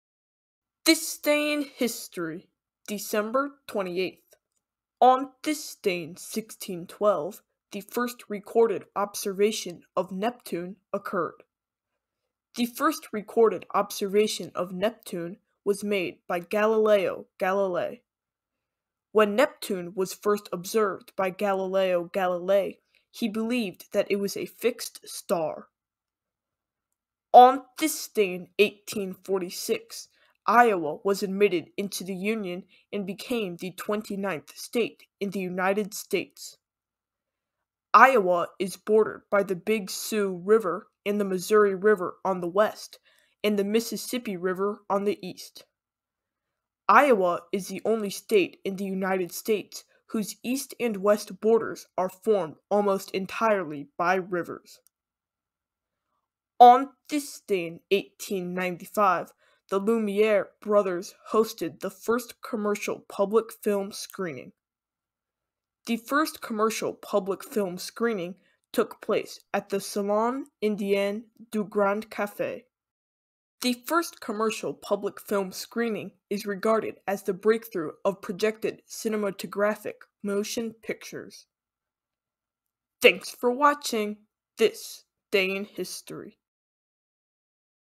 December 28 - This Day in History
This video explains historical events that occurred on December 28.